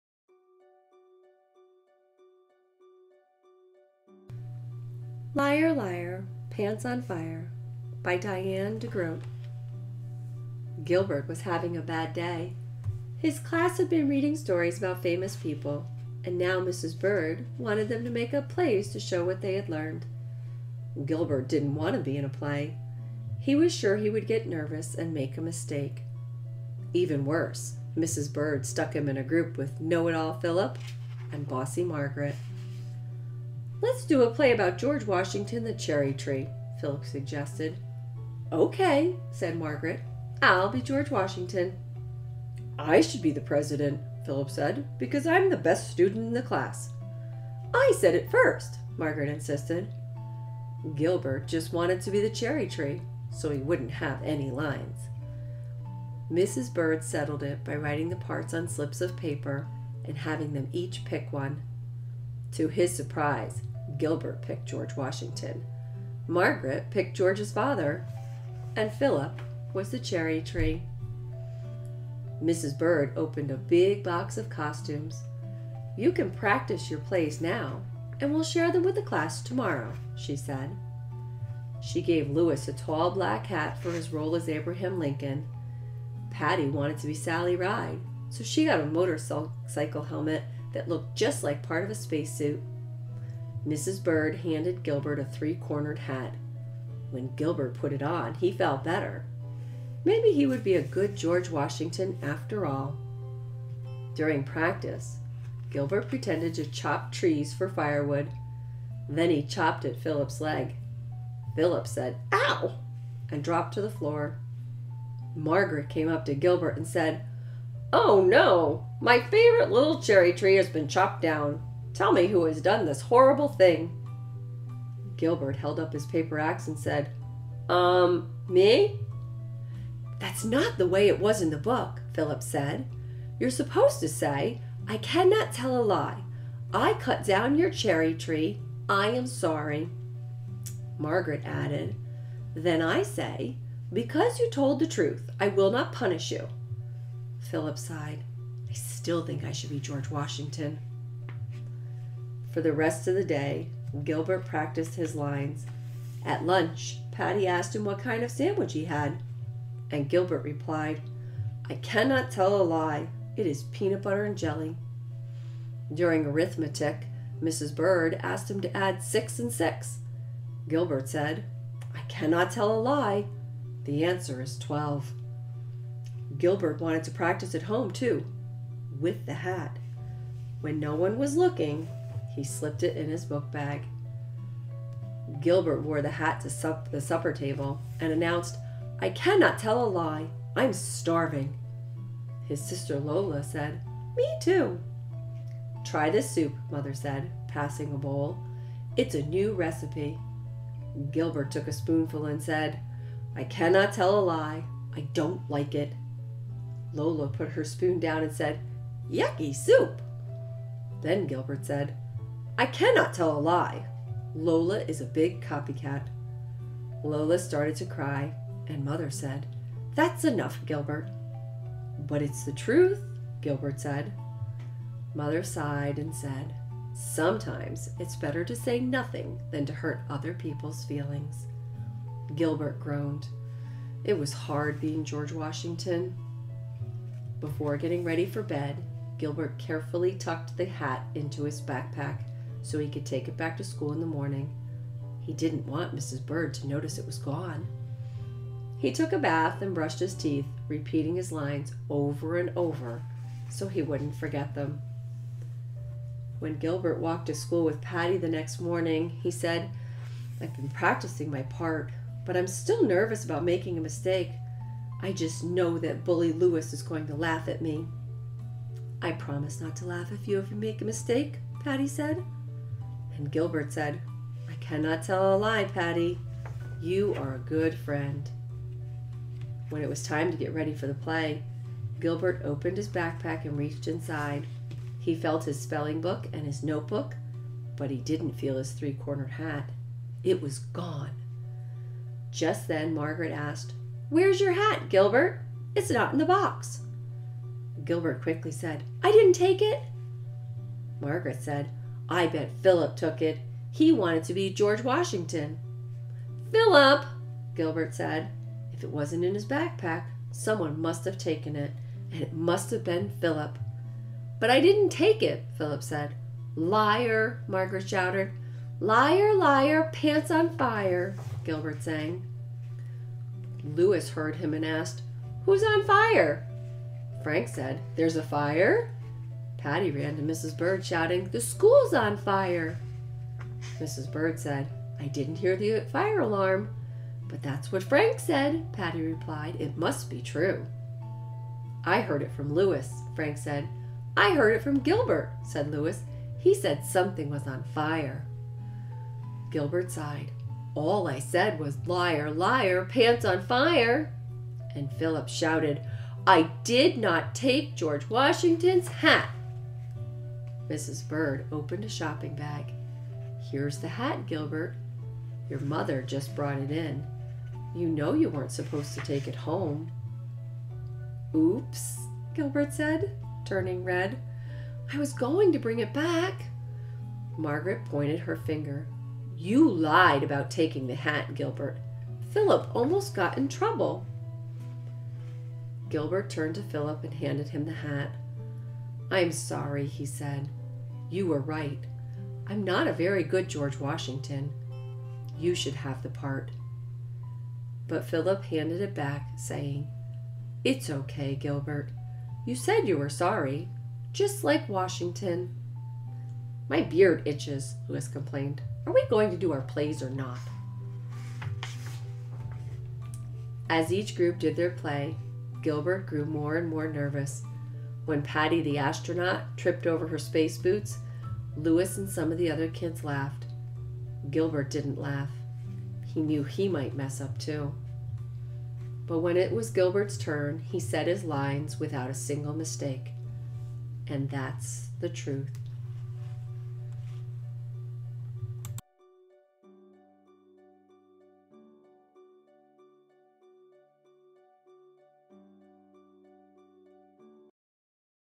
Liar Liar Pants On Fire | Diane deGroat | Presidents Day Children's Book
Gilbert's class is learning all about important people in history. His teacher has assigned student groups to preform  what they have learned for the rest of the class. Gilbert's group chose George Washington and the cherry tree story. Gilbert learns that George Washington always told the truth but when Gilbert did something wrong he finds out how hard it is to be honest. Liar, Liar Pants on Fire by Diane deGroat.